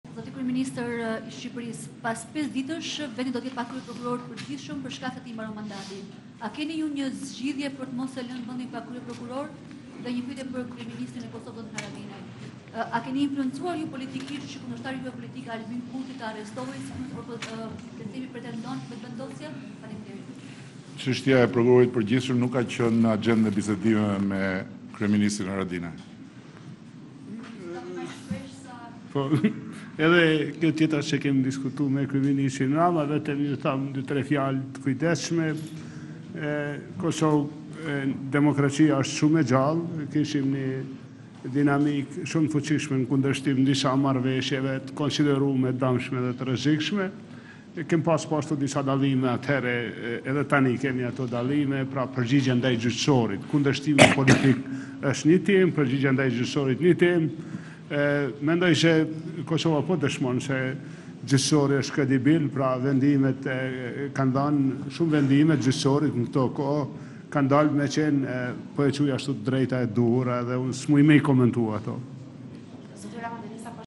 Report TV - Ja si përgjigjet Rama për Kryeprokurorin e ri
Në datë 4 dhjetor Shqipëria do të jetë pa Kryeprokuror, pasi mbaron mandati i Adriatik Llallës.  Përgjegjës për emërimin e kryeprokurorit të ri është KLP-ja, e cila nuk është krijuar.
Shumica kundërshton zgjatjen e mandatit të Llallës, ndërsa demokratet kanë dalë hapur kundër zgjedhjes së një prokurori vetëm me votat e qeverise.

I pyetur nga Report Tv për këtë situatë, kryeministri Edi Rama nuk ka pranuar që ta komentojë duke e shmangur.

Pyetja: Zoti Rama pas pak ditësh vendi do të jetë pa Kryeprokuror, a keni menduar një zgjidhje?

Rama: Kjo çështje nuk ka qenë në axhendën e bisedimeve me kryeministrin Haradinaj

OPDAT dhe EURALIUS kanë dhënë opinion për këtë çështje pak ditë më parë duke theksuar se nuk ka organ në vend që mund të zgjasë mandatin e kryeprokurorit dhe Llalla duhet të largohet.